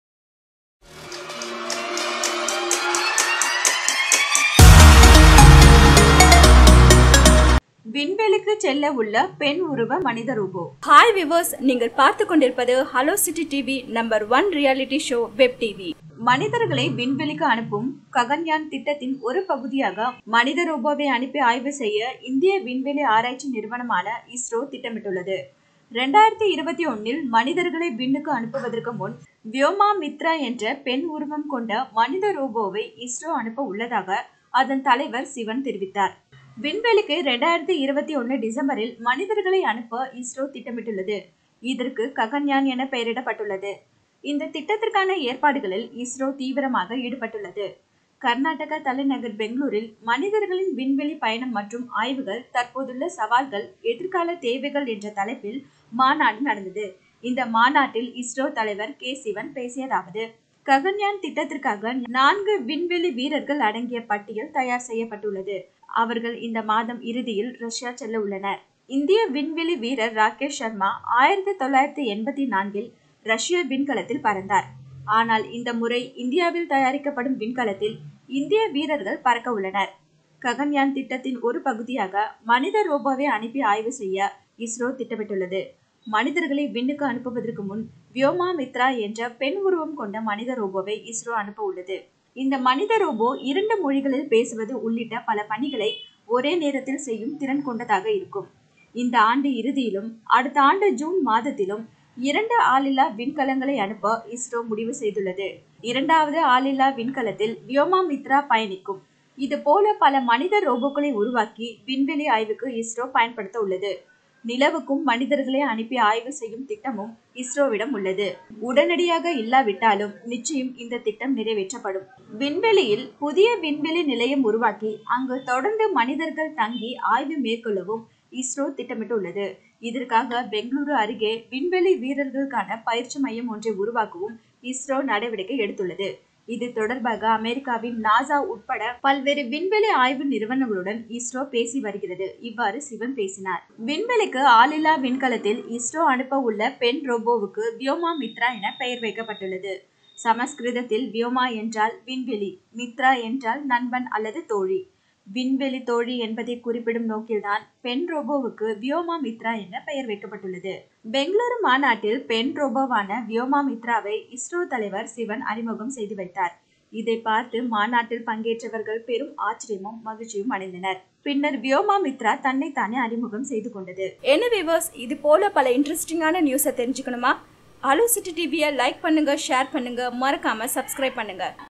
Humoid robot is on the way to space | விண்வெளியில் மனிதர்கள் வாழ முடியுமா? | Tamil News
விண்வெளிக்கு செல்ல உள்ள பெண் உருவ மனித ரோபோ|Space Travel|ISRO|Vyoma Mitra Robo|ISRO Chief sivan